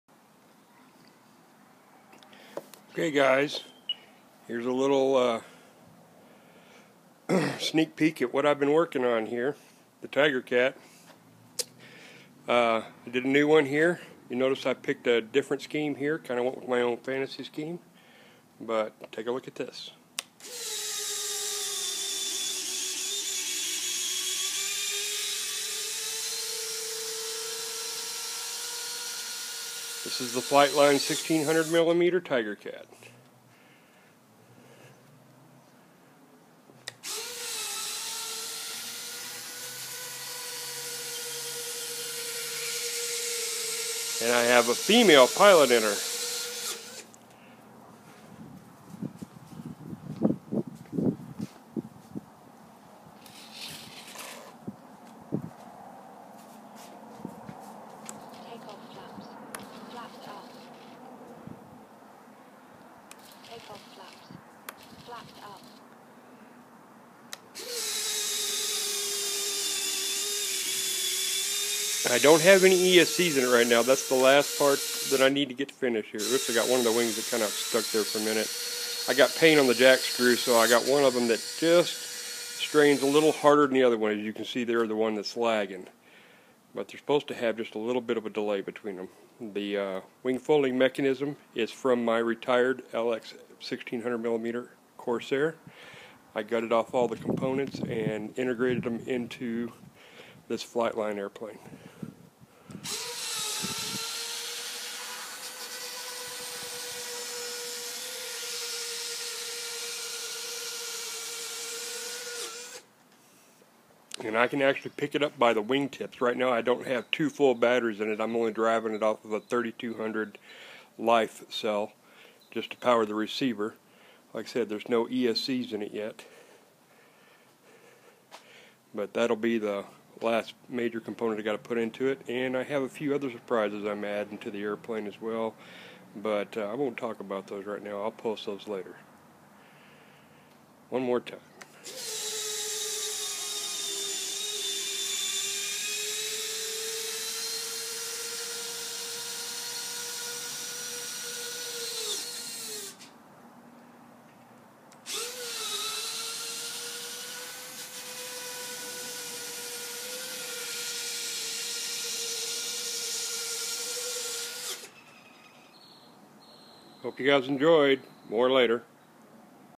Brandon's Modified FlightlineRC 1600mm F7F-3 Tigercat with Folding Wings
She's about 85% complete, still need to add ESC's and finish out the decals and detailing.

The Wings can be locked down with the insertion of a CF tube that inserts from the wing tip to lock them more securely for flight if wanted. did this as a secondary backup precaution and option.

I did the paint job with rattle can spray paints and used a brush to paint the flight surfaces white on the wings.

Update: It is now 100% complete and flight tested. works like a charm ;)

Note: this is NOT an easy modification to do and is not for the everyday modifier. Looks much easier than it actually is.